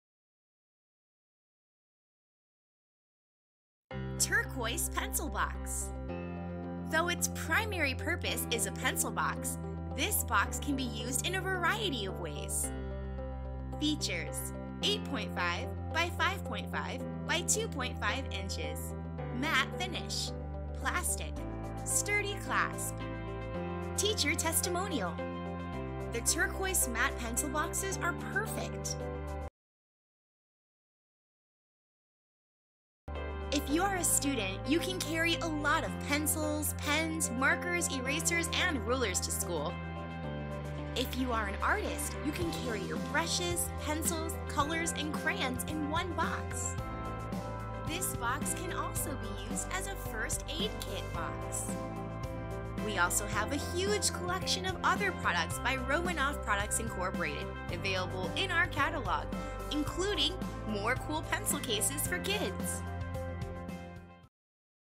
Best Pencil Box - Pencil Case - School Supplies
Buy this versatile turquoise color pencil box manufactured by Romanoff Products Inc. Though it's primary purpose is a pencil box, this box can be used in a variety of ways!

The Romanoff Products Inc. is a leading name in the school supplies industry, and produces quality products for everyone to enjoy. The pencil case is a big storage box with a hard cover. Its hard clasp protects all of your stored items perfectly. If you are a student, you can carry a lot of pencils, pens, markers, erasers, ruler etc. to school. If you are an artist, you can carry your brushes, pencils, colors, and crayons in one box. Or, you can also carry your craft tools like scissors, thread and so much more. This box can also be used as a first aid kit box. Kids can even store their tiny toys, puzzle pieces, dice, etc. in the box. This product can be used in various ways according to your own personal needs!

The pencil and pen case has the following features:
Roomy dimensions of 8.5" X 5.5" X 2.5"
Turquoise with a matte finish
High-quality plastic material
Sturdy clasp

We also have a huge collection of other products by Romanoff Products Inc. available in our catalog, including more cool pencil cases for kids.